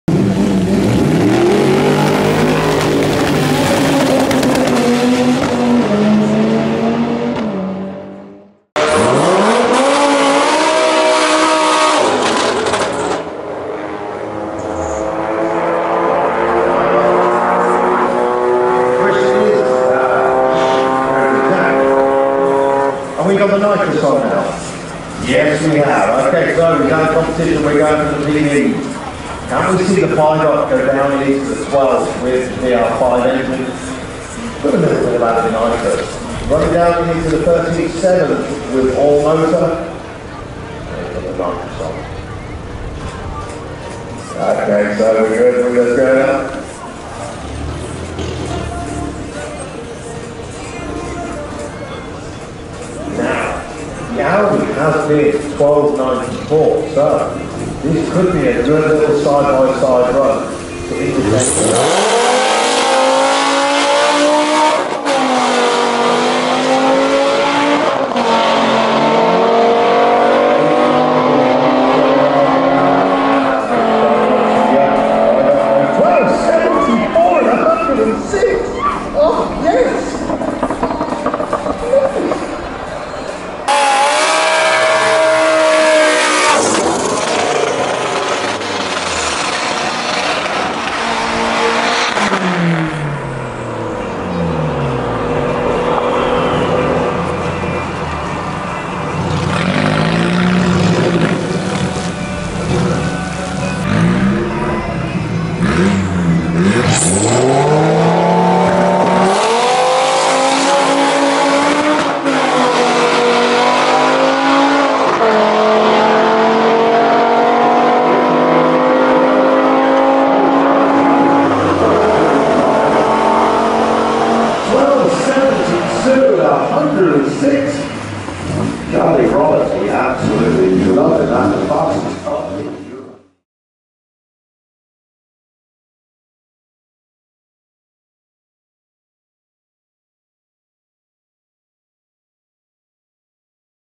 QUICKEST VW UP IN EUROPE - V5UP - 12.72 @ 106mph
Charlie Roberts was at the VW Breakout Show at Santa Pod Raceway in his VW UP, which is fitted with a VW 2.3l V5 Golf and gearbox, which produced 200bhp on the rollers at EST Performance. He has been running in the mid 13's on this setup, but with the added help of a 75hp shot of NOS, he broke into the 12's with the quickest run being a 12.72 @ 106mph. This time I have been told, makes it the quickest VW UP in Europe. Does anybody know otherwise?

Also check out their YouTube channel for updates on the latest car mods, and workshop videos